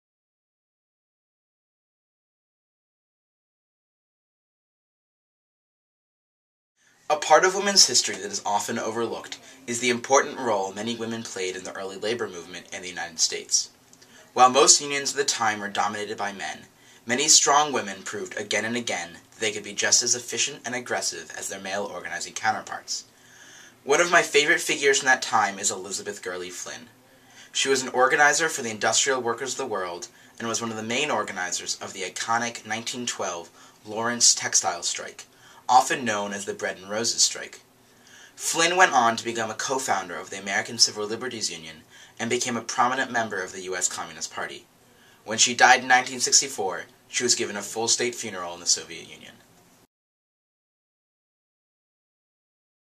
WHM Fact 2: Women in Labor?
Women's Initiative Director of Men's Outreach Ethan Miller gets real about women in the history of the labor movement.